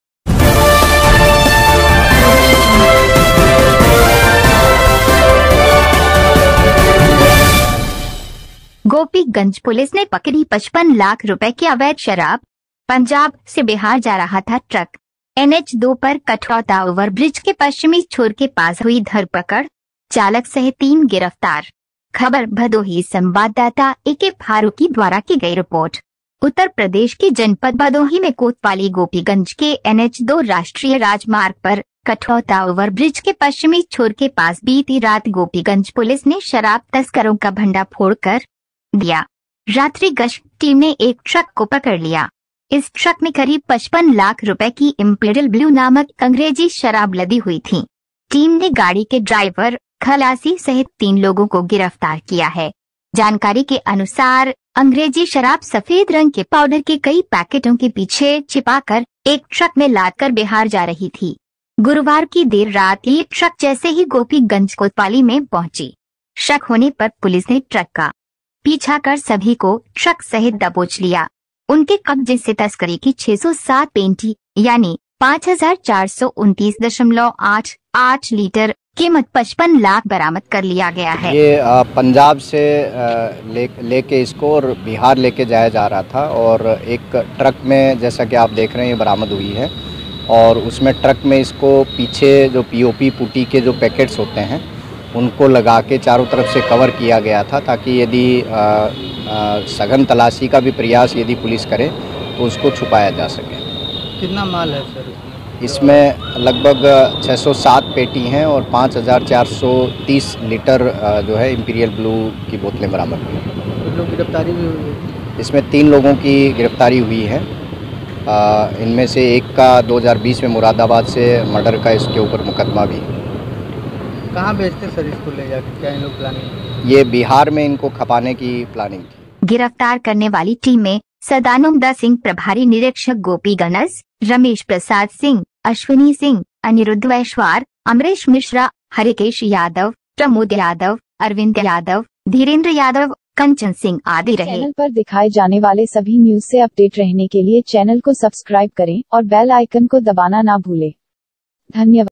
गोपीगंज पुलिस ने पकड़ी 55 लाख रुपये की अवैध शराब!पंजाब से बिहार जा रहा था ट्रक!
एन एच 2 पर कठौता ओवर ब्रिज के पश्चिमी छोर के पास हुई धरपकड़!
चालक सहित तीन गिरफ्तार!
  खबर भदोही संवाददाता ए के फारूकी द्वारा कि गई रिपोर्ट!
   उत्तर प्रदेश के जनपद भदोही में कोतवाली गोपी गंज के एन एच 2 राष्ट्रीय राजमार्ग पर,
   कठौता ओवर ब्रिज के पश्चिमी छोर के पास बीती रात गोपी गंज पुलिस ने शराब तस्करों का भंडाफोड़ कर दिया!
  रात्रि गश्त  टीम ने एक ट्रक को पकड़ लिया!
  इस ट्रक में करीब 55 लाख रुपये की इम्पीरियल ब्लू नामक अंग्रेजी शराब लदी हुई थी!
   टीम ने गाड़ी के ड्राइवर, खलासी सहित तीन लोगों को गिरफ्तार किया है!
   जानकारी के अनुसार, अंग्रेजी शराब सफेद रंग के पाउडर के कई पैकेटों के पीछे छिपाकर,
   एक ट्रक में लादकर बिहार जा रही थी! गुरुवार की देर रात यह ट्रक जैसे ही गोपी गंज कोतवाली में पहुंची!
   शक होने पर पुलिस ने ट्रक का पीछा कर सभी को ट्रक सहित दबोच लिया!
   उनके कब्जे से तस्करी की 607 पेंटी,
 यानि,(5429.88 लीटर), कीमत 55 लाख बरामद कर लिया गया है!
   पूछताछ के दौरान मुरादाबाद जनपद के बिल्लारी थानाक्षेत्र के रुस्तमपुर निवासी मलखान सिंह और,  राजेंद्र मौर्य! तथा,
   इब्राहिम पुर मिर्जा निवासी अमरीष मौर्य ने संयुक्त रूप से बताया कि,
   मोटर मालिक अभय सिंह इस ट्रक पर अवैध अंग्रेजी शराब रखवा कर मुरादाबाद तक लाए थे! और,
   वहां से हम लोगों को अवैध शराब लदी ट्रक को बिहार ले जाने के लिए दिया था!
   ट्रक मालिक अभय सिंह द्वारा अवैध शराब को लदवा कर बिहार सहित, विभिन्न राज्यों को भेजा जाता है!
   जिसको मैं चालक मलखान सिंह पैसों का लेन-देन करता हूं! और, ट्रक मालिक अभय सिंह सहित,
   मेरे साथ काम कर रहे सहयोगी, अमरीश मौर्य व, गजेंद्र को वितरित करता हूं!
    हम लोग अवैध शराब तस्करी ट्रक का  नंबर बदल बदल कर करते हैं!
   हम लोगों का अवैध शराब तस्करी का एक सुसंगठित गिरोह है! जिसके अर्जित धन से,
   हम लोगों को आर्थिक भौतिक व, दुनियाबी लाभ होता है!
   इस बार हम तीनो लोग मुरादाबाद से बिहार अवैध अंग्रेजी शराब बेचने जा रहे थे कि, पुलिस द्वारा पकड़ लिए गए!
   गिरफ्तार करने वाली टीम में, सदानं द सिंह प्रभारी निरीक्षक गोपीगंज, रमेश प्रसाद सिंह, अश्विनी सिंह, अनिरुद्ध वैशवार,
   अमरेश मिश्रा, हरिकेश यादव, प्रमोद यादव, अरविंद यादव ,धीरेंद्र यादव, कंचन सिंह आदि रहे!